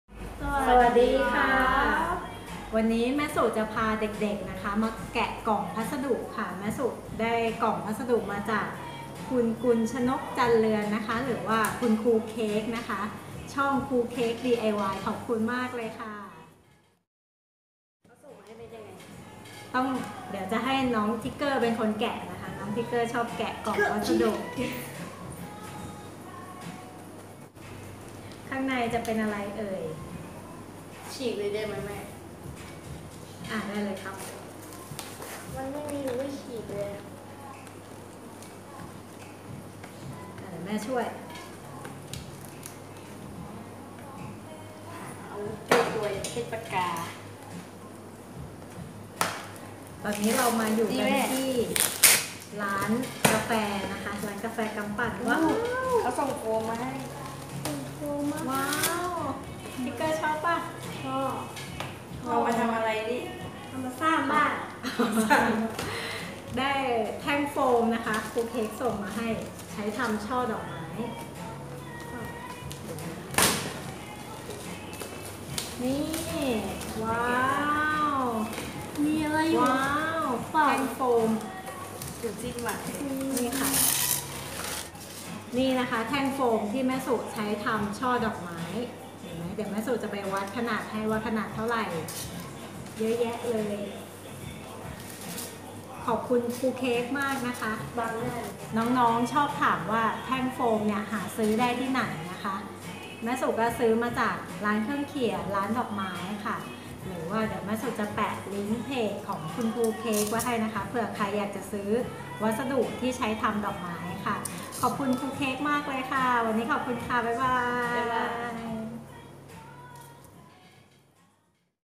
แกะกล่องพัสดุ จาก ครูเค้ก | DIY ง่ายนิดเดียว
ขอบคุณ ครูเค้ก diy crafts ที่ส่งของมาให้แม่สุ สั่งซื้ออุปกรณ์ดอกไม้ประดิษฐ์ได้ที่ เพจครูเค้ก นะคะ